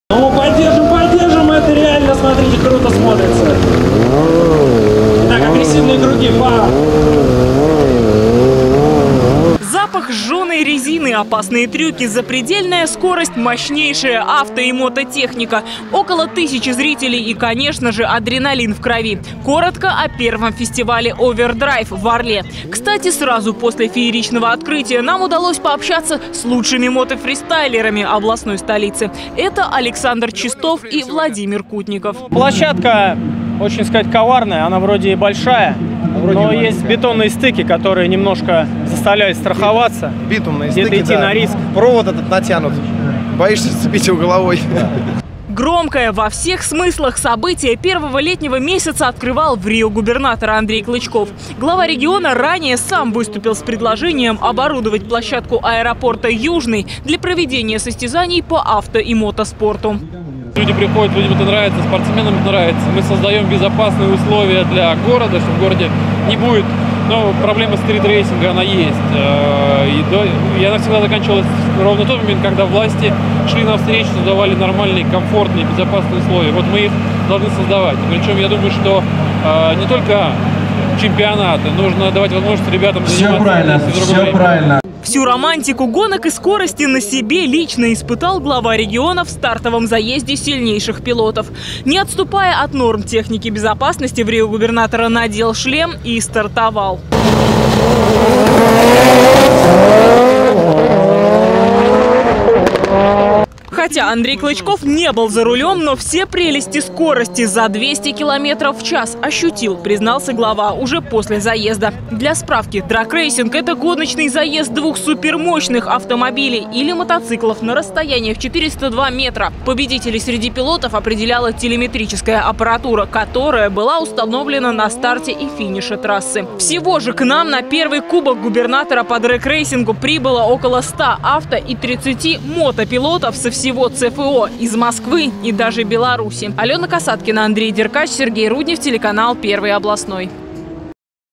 В Орле состоялись состязания на Кубок губернатора по дрэг-рейсингу
На площадке аэропорта "Южный" фестиваль скорости "Овердрайв" собрал около 130-ти авто- и мото- любителей.